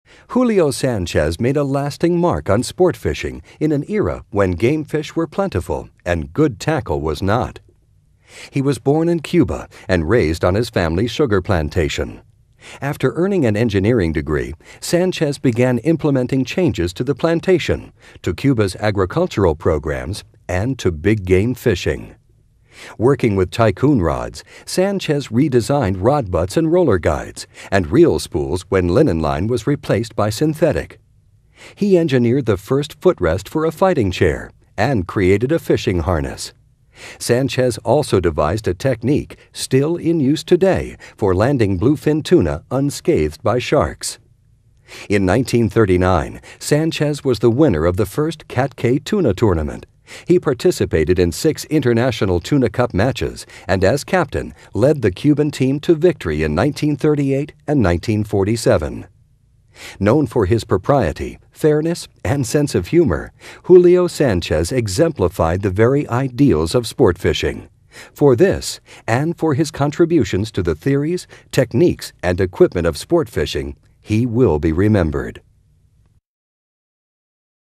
Julio Sanchez - IGFA Fishing Hall of Fame
Julio Sanchez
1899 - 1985
2001 Inductee
 Julio Sanchez made a lasting mark on sportfishing in an era when game fish were plentiful and good tackle was not. He was born in Cuba in 1899 and raised on his father's sugar plantation, where at a young age he hunted game and fished for tarpon in the Rio Encantado. After earning a civil and electrical engineering degree at Rensselaer Polytechnic Institute, Sanchez began making fundamental changes to his family's plantation, to many of Cuba's farm and flood control programs, and to the theories, techniques and equipment of big-game fishing. As unofficial field tester for Tycoon Rods, he proposed lengthening the rod butt, thus enabling the angler to increase his leverage, his pull, and his odds for landing the big ones. As a result of his input, ball bearing roller guides were added to Tycoon Tackle's famous Bimini King rod, a favorite of big-game fishermen. Sanchez was the first to increase the thickness of the reel spool when synthetic fishing line began replacing linen; designed the first footrest for a fighting chair, allowing anglers to remove their feet from the transom; created a fishing harness that was a precursor of the bucket seat; and devised a fishing technique for landing bluefin tuna unscathed by sharks which is still in practice today. Sanchez, together with Ernest Hemingway, pioneered big-game fishing in the Bahamas; was a founding member of the Miami Beach Rod and Reel Club in 1930; won the first Cat Cay Tuna Tournament in 1939 with a total bluefin catch of 4,177 pounds; and participated in six International Tuna Cup Matches at Wedgeport, Nova Scotia, leading the Cuban Team to victory in 1938 and 1947. A modest man known for his propriety, fairness and sense of humor, Julio Sanchez will be remembered for his ingenuity and for exemplifying the very ideals of sport fishing: sportsmanship, good will and mutual understanding between countries.